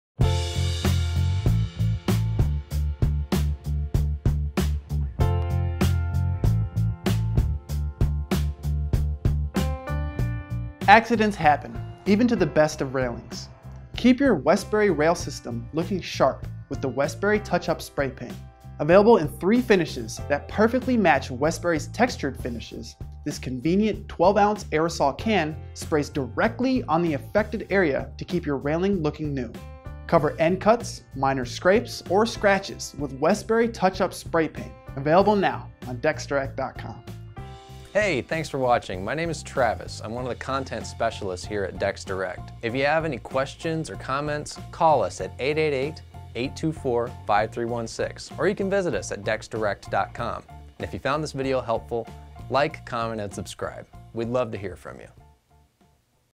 Touch Up Spray Paint by Westbury Aluminum Railing
Cover end cuts, minor scrapes or scratches in your Westbury Rail System with this handy Touch Up Spray Paint. Available in a convenient 12 oz. aerosol can, it comes in three finishes that perfectly match Westbury's textured finishes.